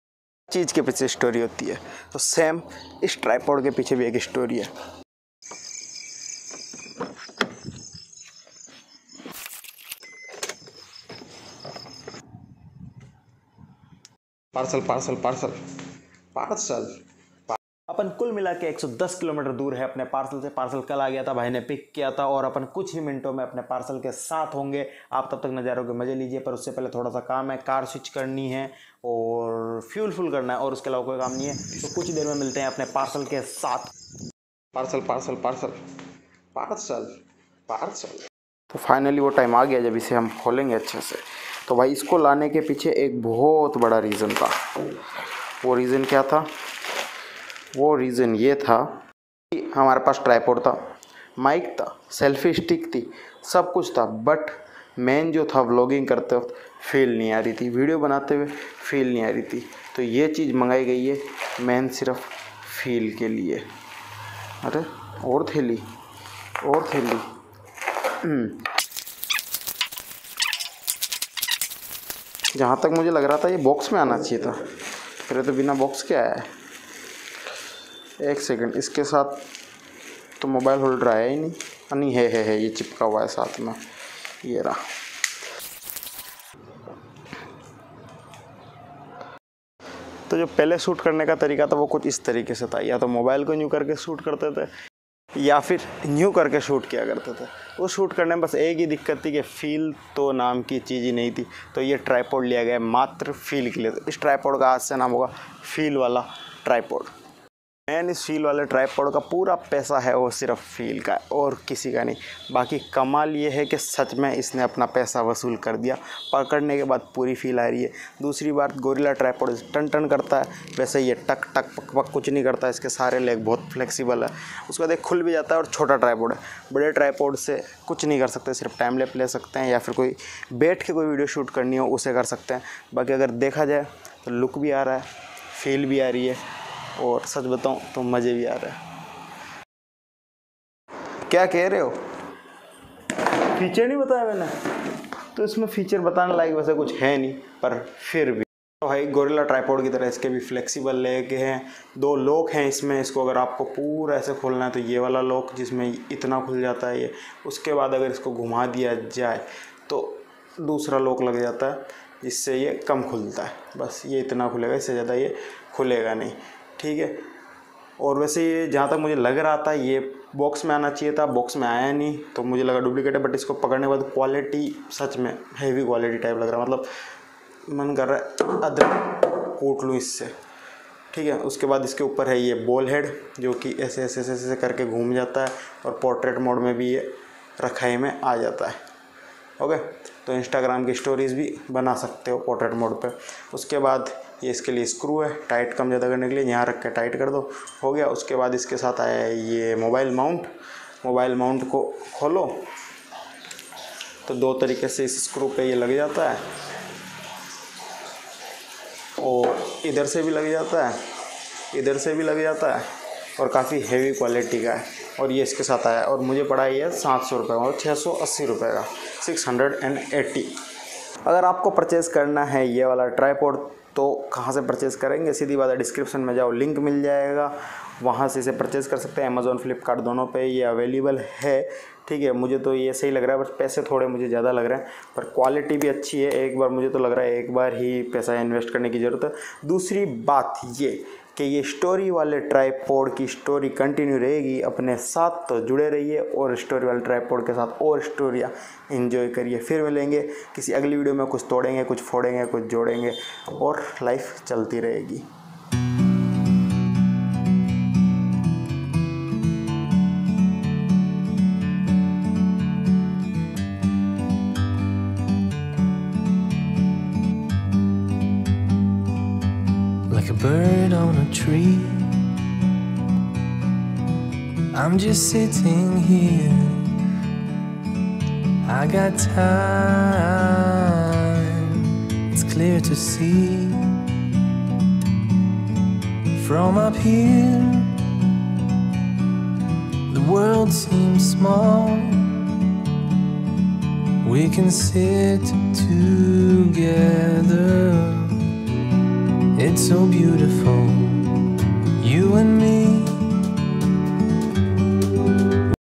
Feel wala Tripod | Best Tripod for vlogger | Adofys Flexible Gorillapod Tripod Rs. 800
Best Budget gorillapod tripod for vlogger and beginner.